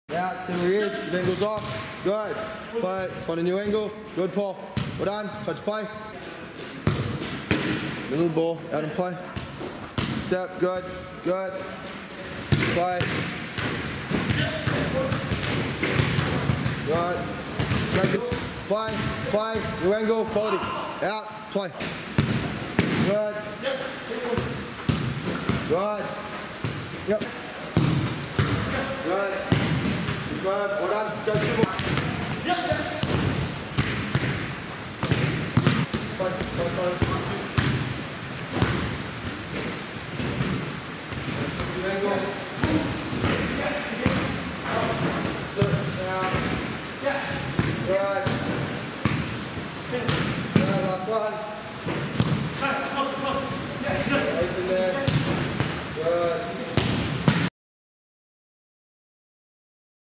Midfielder Position Specific Drill: Combination play with two Pro players
Lebanese National Team player Yahya El Hindi and Paul Katsetis putting on a show in this passing drill for midfielders. Yahya is a defensive midfielder and Paul is an attacking midfielder, so this drill worked on Yahya receiving the ball and facing forward with his first touch with Paul then having to give an angle off the ball to pass to.